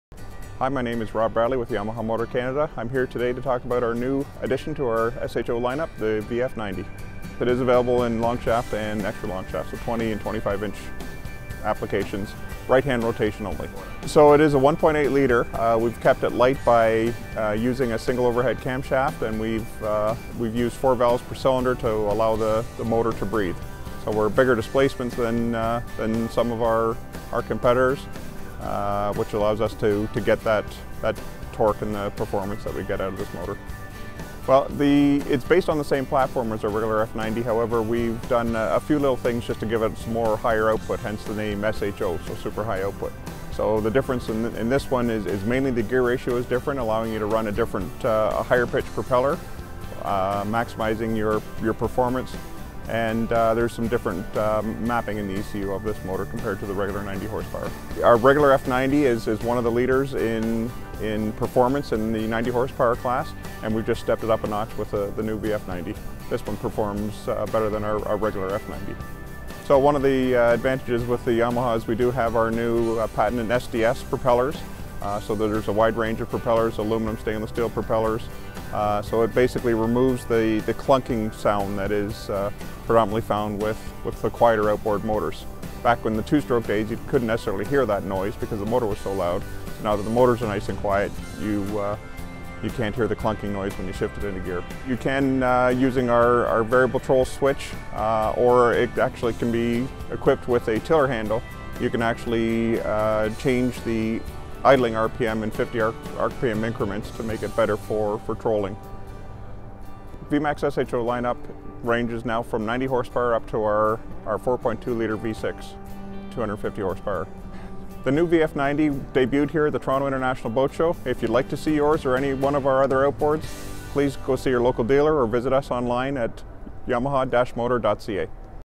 In depth on the Yamaha VF90 at the Toronto International Boat Show
This segment was featured in an episode of the season.